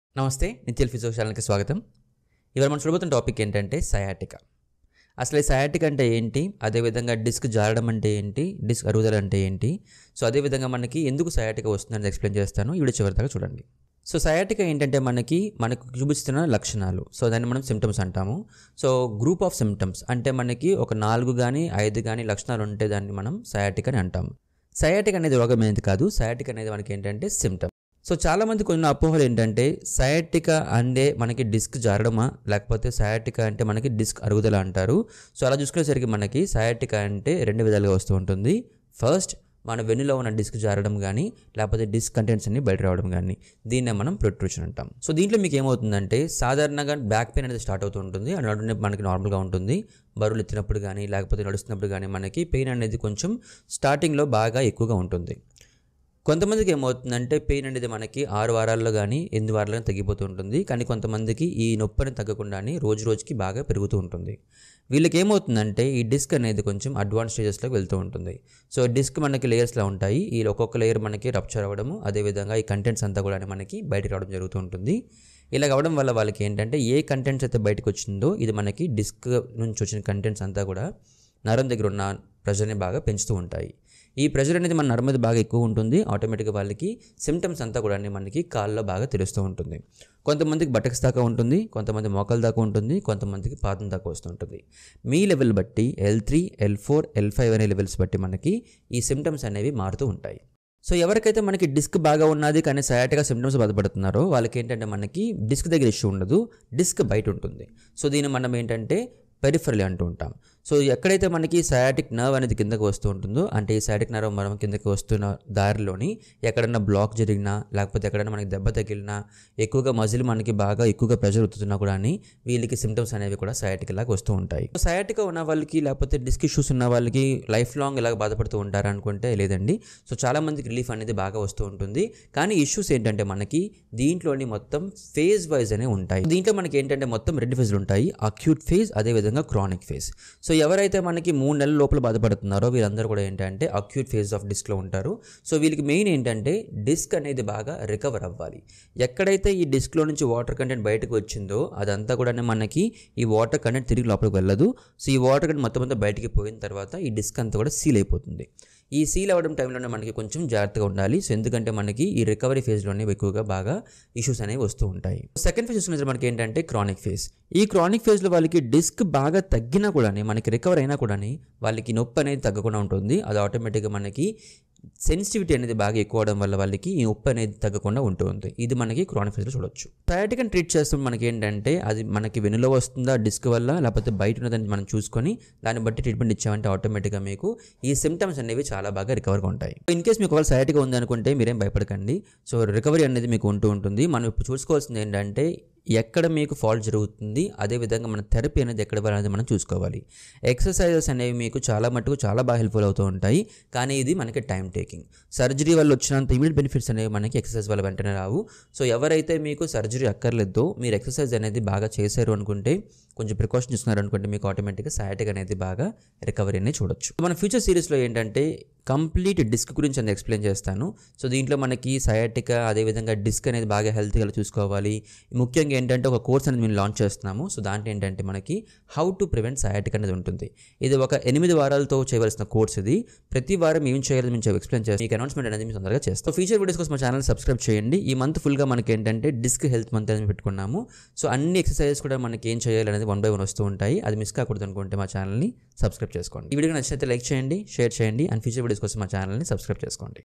What is Sciatica?
In this video Dr. Nityal Kumar from Nityal Physio has explained on what is Sciatica?